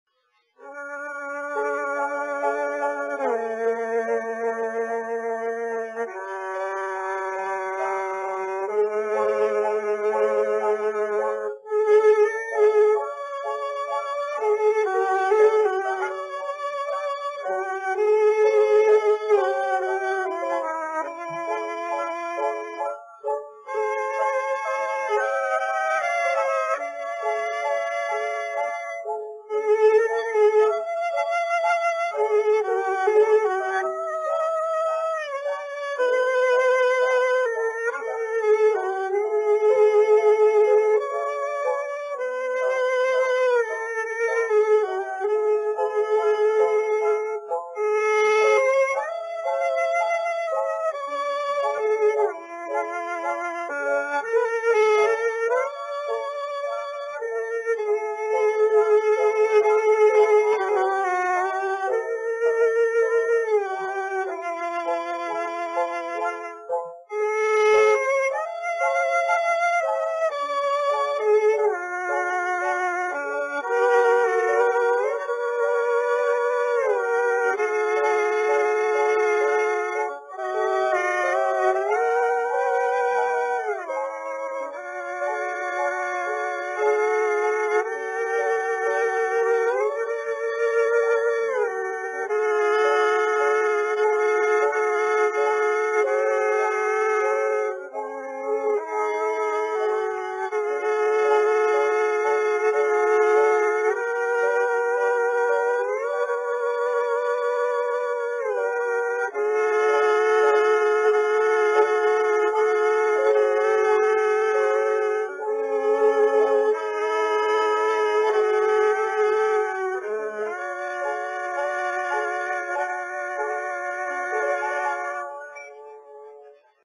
Canoeing in Boston 1904
Vintage video from Thomas Edison collection with classical piece all from 1904